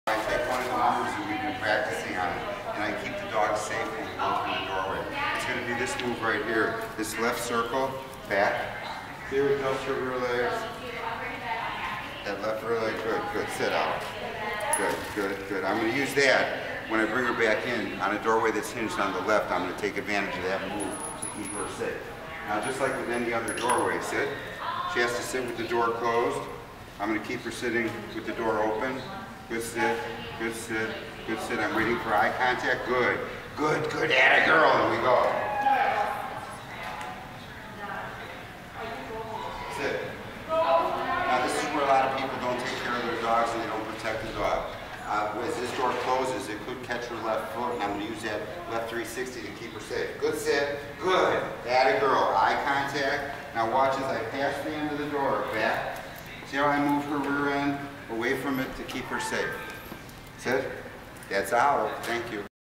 Allan Ross Dog Training
Olive N. is a 1.2 year old Lab mix in her first week of Board and Train.  Here we demonstrate getting eye contact and protecting Olive from a door hinged on the left.